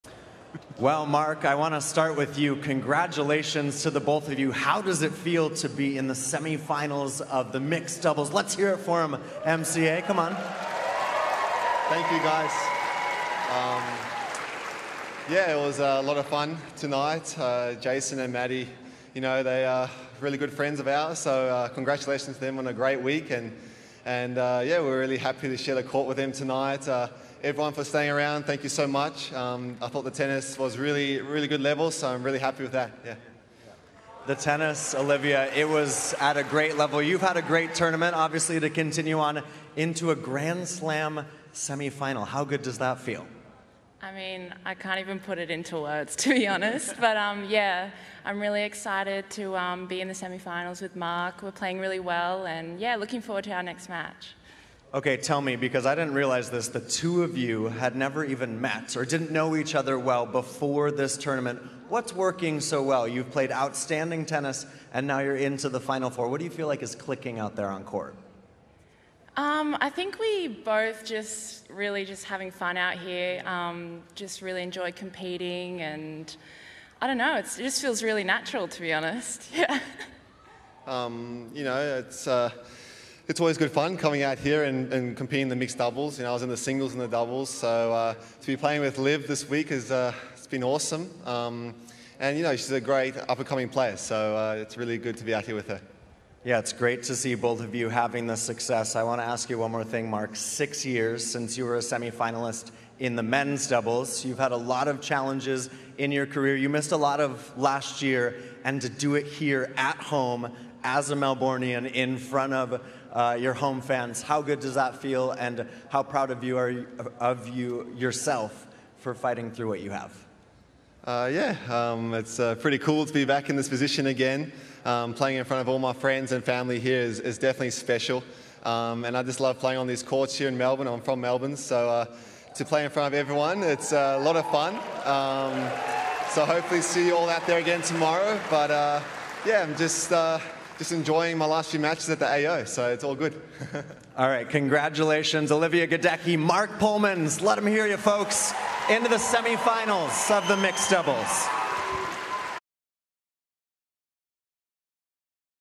Gadecki/Polmans On-Court Interview | Australian Open 2023 Quarterfinal
Gadecki/Polmans’ on-court interview following their win against Inglis/Kubler in the quartefinals of the Australian Open 2023.